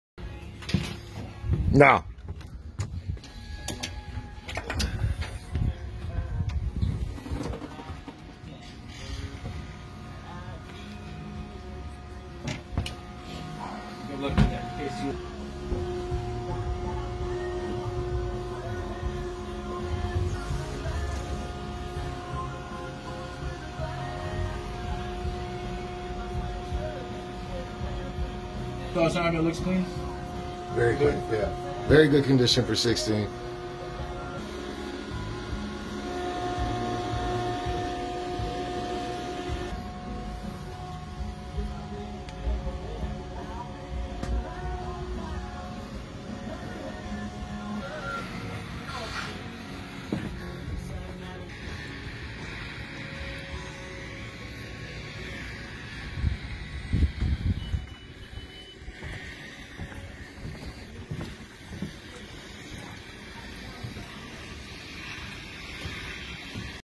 2016 SeaVee 320z
2016 Seavee 320z
640 hours
Twin 350 verados
Cruise is 40-45 mph
1.4 mpg
Top speed 57-61mph
1 tanks 310 gallons
Range is around 400